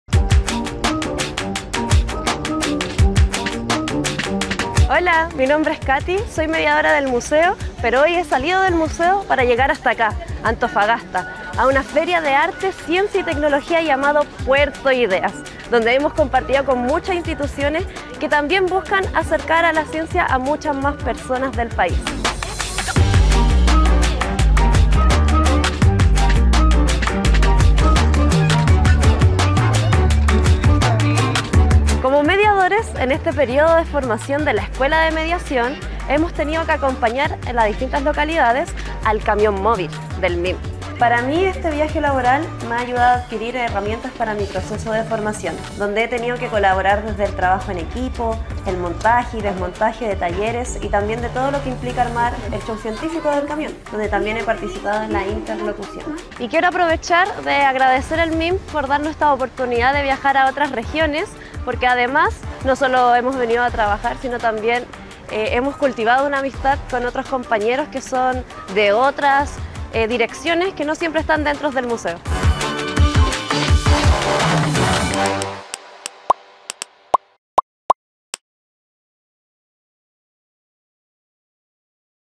Escuela de Mediación en Puerto Ideas
En Puerto de Ideas 2023, una de las mediadoras de la Escuela de mediación MIM realizó actividades educativas a distintos públicos del Festival, como parte de su proceso formativo.

👉La Escuela de Mediación es una instancia formadora que se compre de una parte práctica, en contacto permanente con las audiencias y otra teórica, donde adquieren conocimiento y habilidades definidas por el programa de formación para el oficio de la mediación cultural en museo. Este programa es diseñado por la Dirección de Educación del MIM.

📌Las y los jóvenes como Katherine Morales, iniciaron su proceso formativo en septiembre del 2022 y nos acompañarán durante un año. 👏Felicitamos a Katherine por todo su trabajo realizado en Antofagasta.